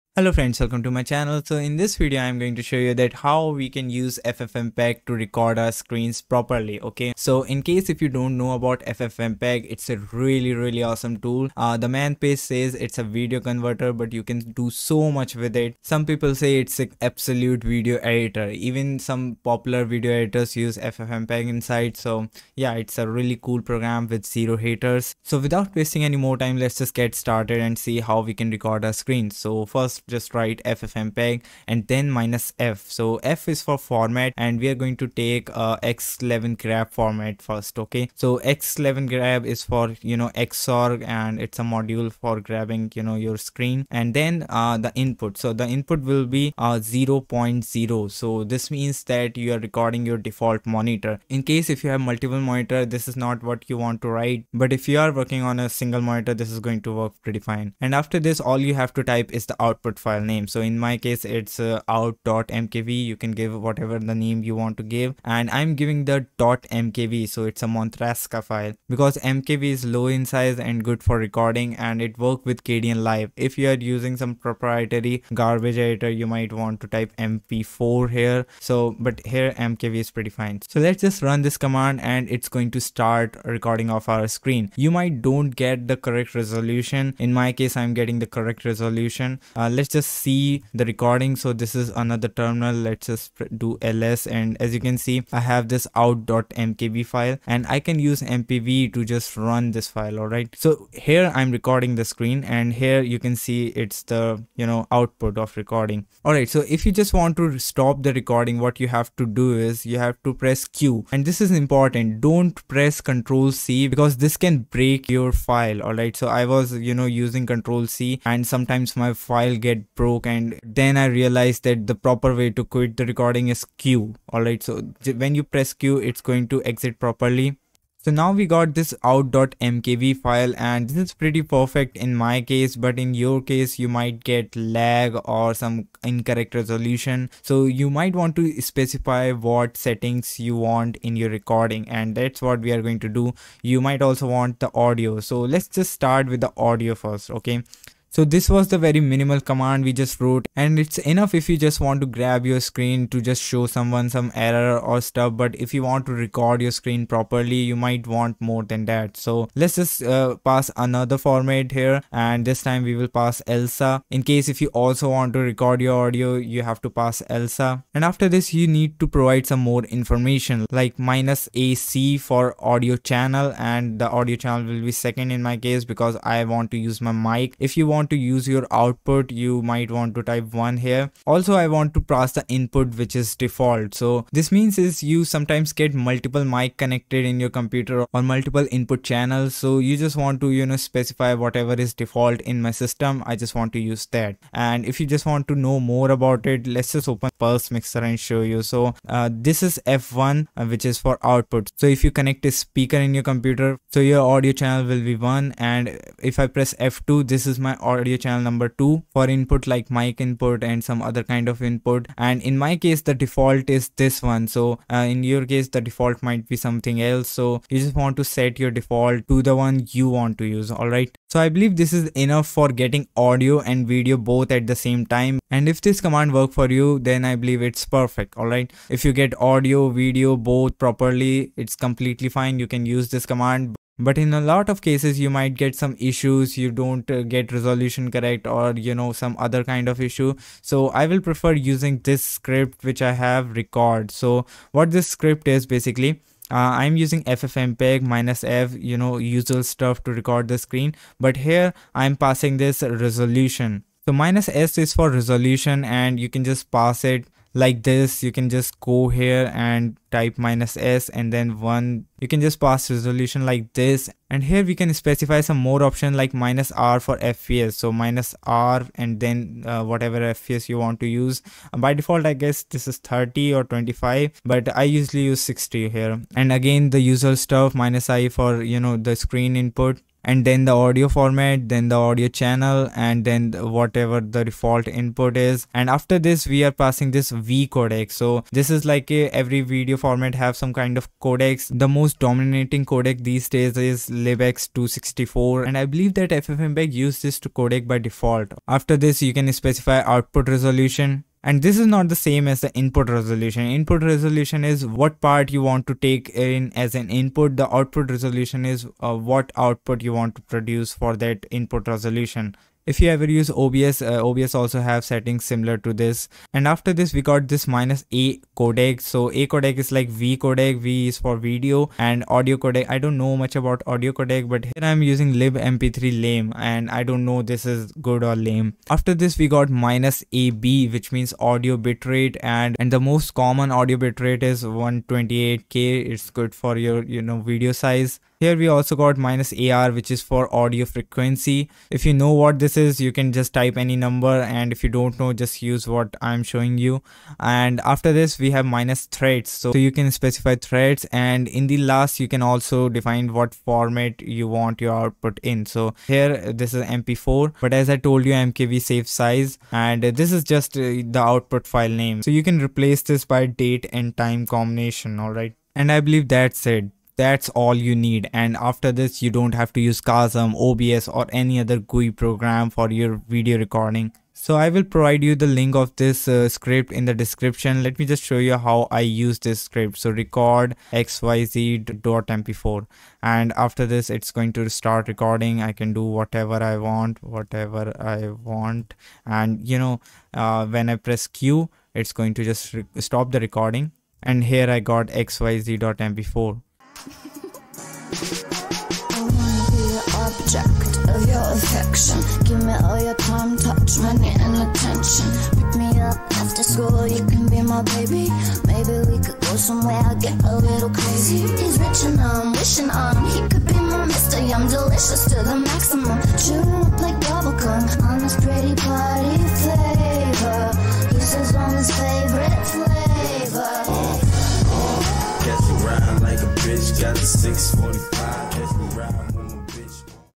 Stop using kazam/obs GUI tools, record screen with ffmpeg
How to screencast with ffmpeg properly.
I believe ffmpeg is enough since this is what obs use, so why to use a GUI program when you can just use ffmpeg simply.